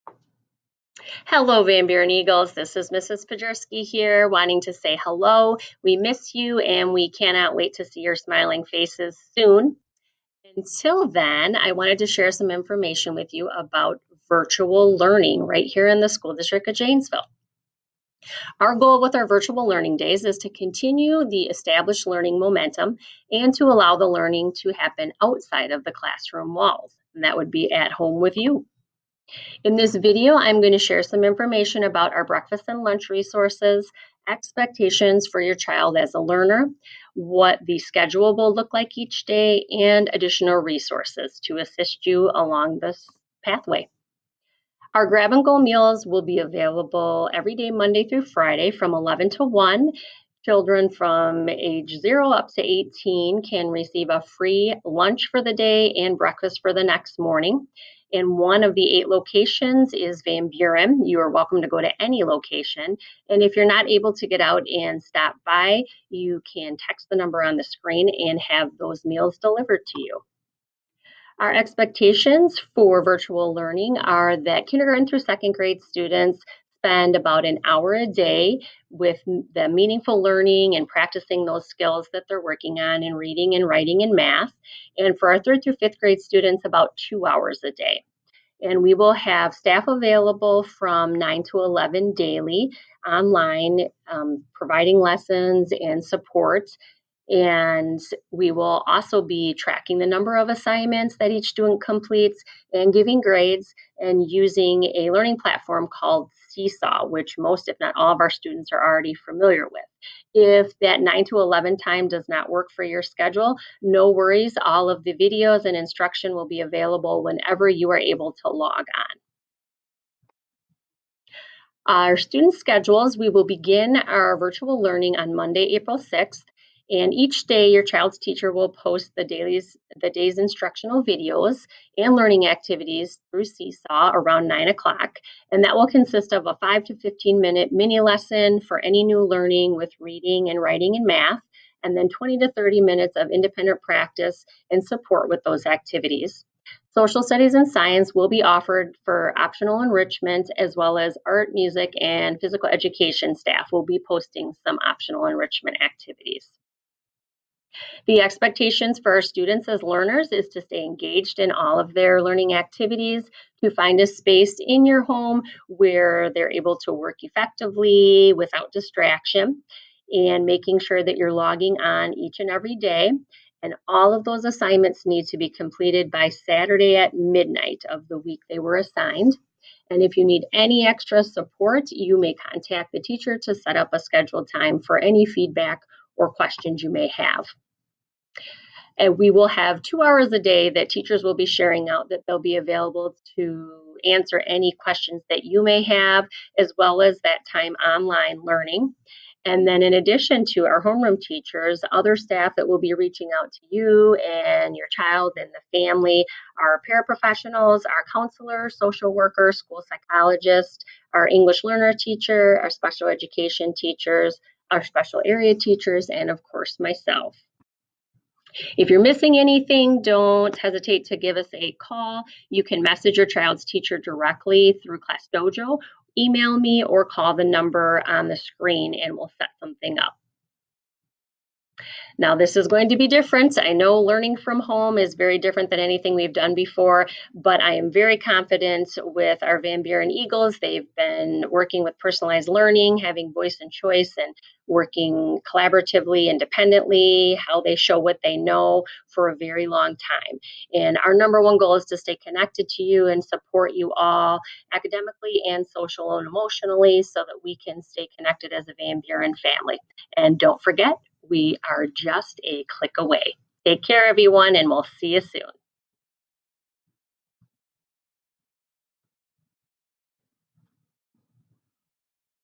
Van Buren Virtual Learning intro-- March 2020
Information for Van Buren Elementary School parents regarding online virtual learning for their children.